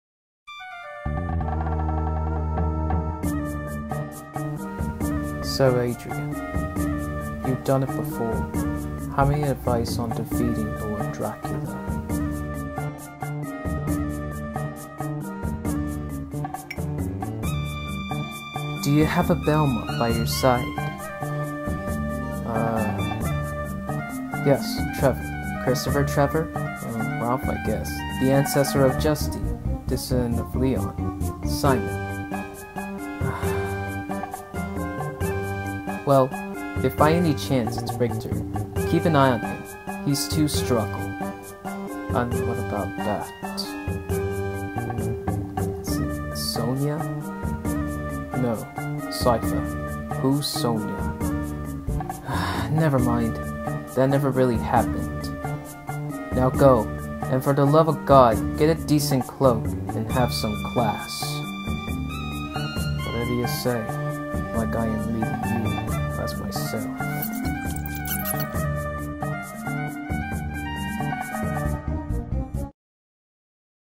Castlevania SOTN+ castlevania Netflix
This my first comic dub I’m doing been wanting to do it so lucky I got cool comic we’re Netflix Alucard meets SOTN alucard hope you like it it’s my first time big fan of castlevania

Comic made by

Alga ~this bat is lightish red